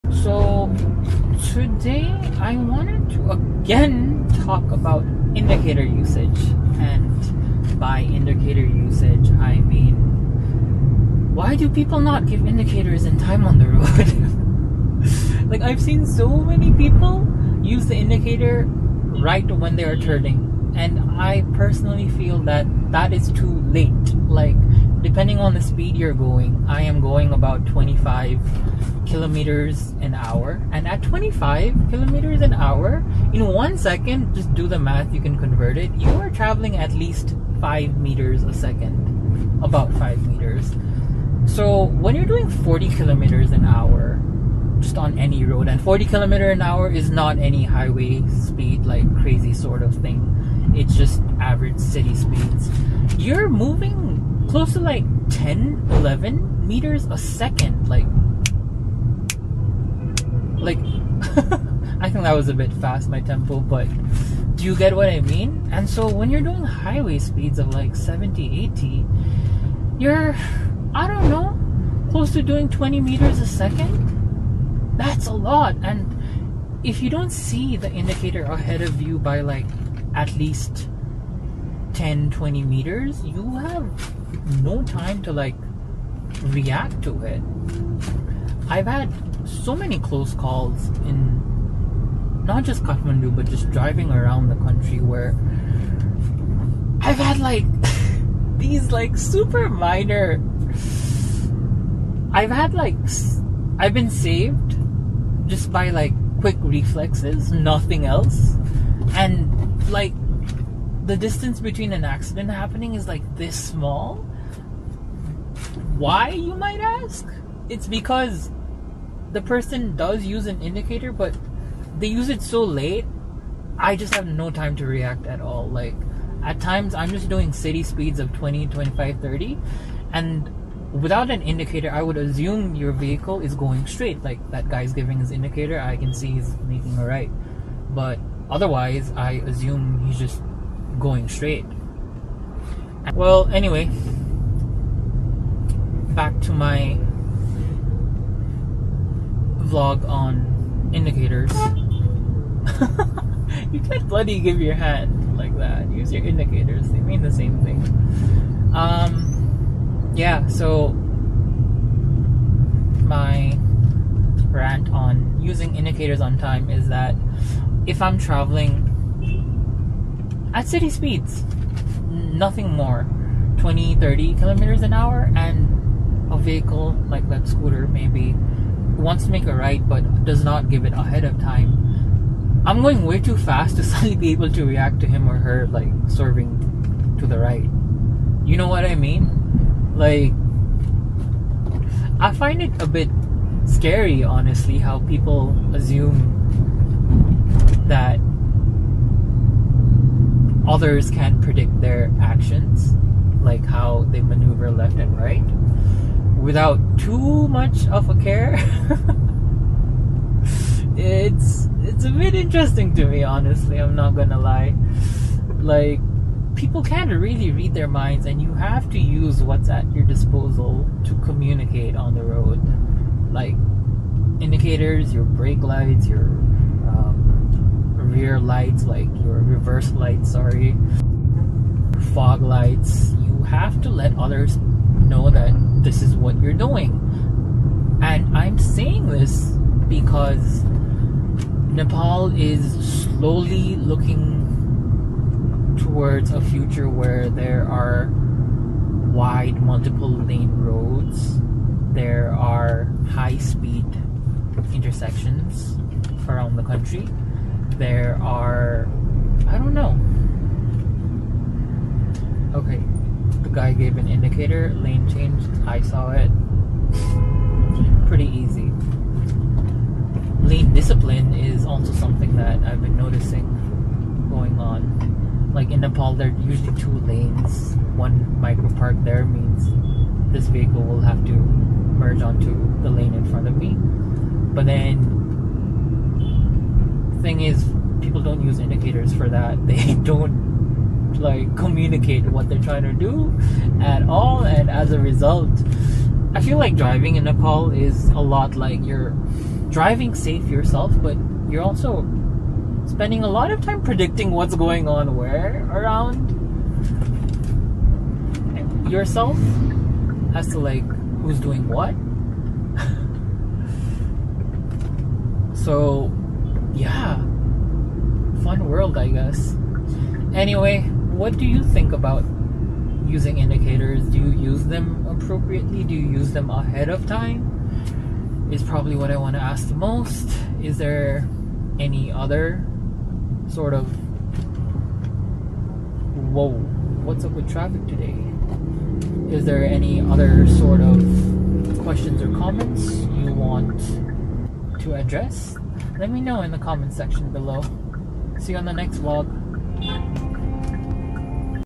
Vlog # 30: Indicator rant (AGAIN!)
Do you know how fast you’re traveling every second when you go city speeds of 25kph and what delayed indicators can cause for others? Check it out!

-CAD